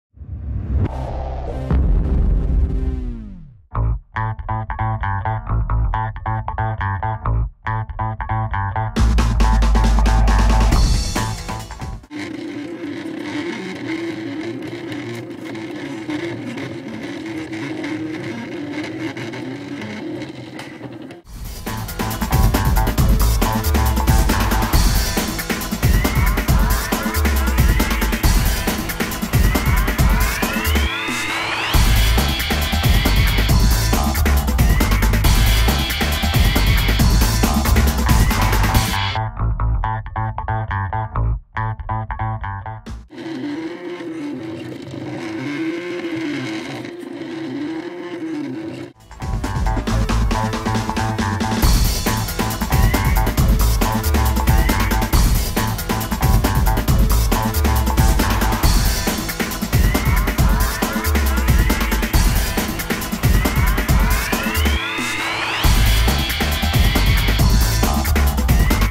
Sonaer Variable Spray Shaping Atomizer Nozzle
How Sonaer's Variable Spray Shapping Atomizer Nozzle was made